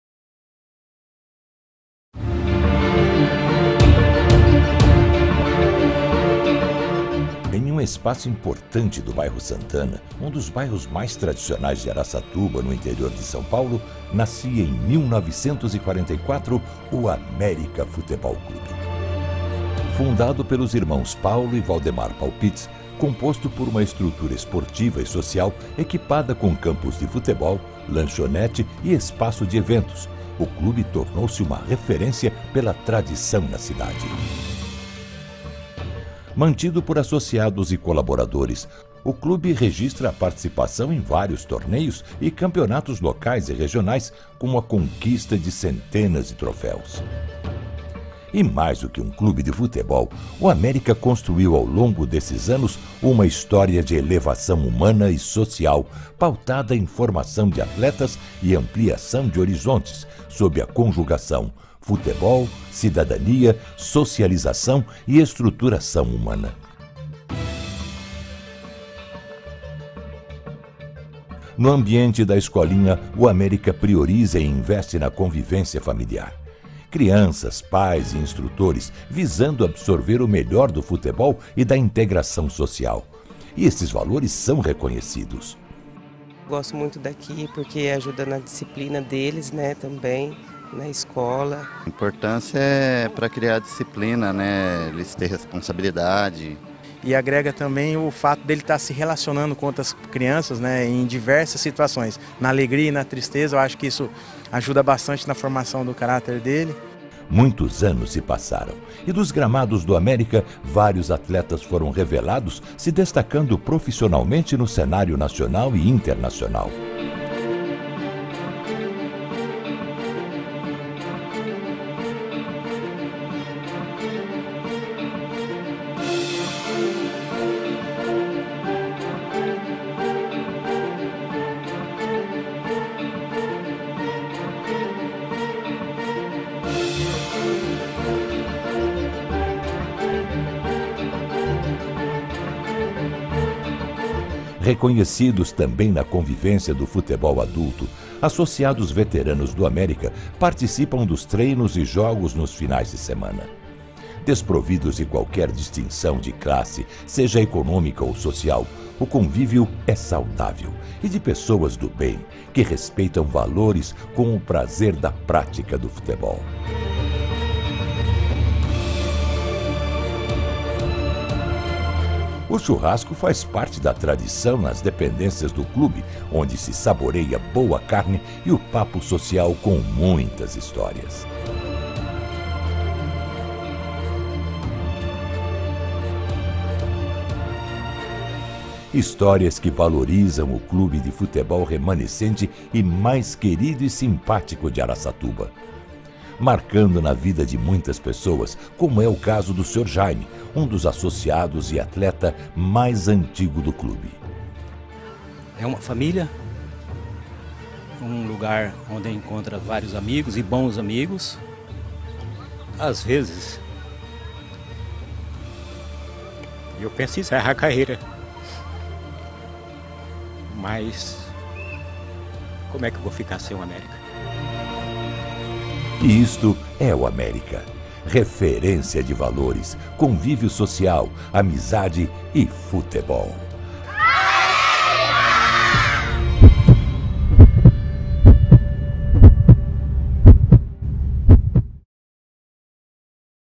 AMÉRICA FUTEBOL CLUBE ARAÇATUBA
"O LEÃO DO SANTANA"

VÍDEO EM HOMENAGEM A ESTE CLUBE QUERIDO DA CIDADE

TIME QUE TAMBÉM REVELA JOGADORES PARA O MUNDO COM:

ALLAN SOUZA - LIVERPOOL ING. / SJK SEINAJOKI FIN. / ST. TRUIDEN BEL. / HERTHA BERLIN ALE. / APOL. LIMASSOL CIP. / FRANKFURT ALE. / FLUMINENSE BR

VINÍCIUS LIMA - GREMIO BR. / RCD MALLORCA ESP. / CEARÁ SC BR / AL-WASL EAU.

XANDÃO - GUARANI BR. / DESP. BRASIL BR. / FLUMINENSE BR. / PRUDENTE BR.  / SÃO PAULO BR. / SPORTING CP POR. / KUBAN KRASNODAR RUS. / ANZHI RUS. / REAL SPORTING RSP. / CERCLE BRUGGE BEL. / RED BULL BR. / PERSIJA JAKATA IND.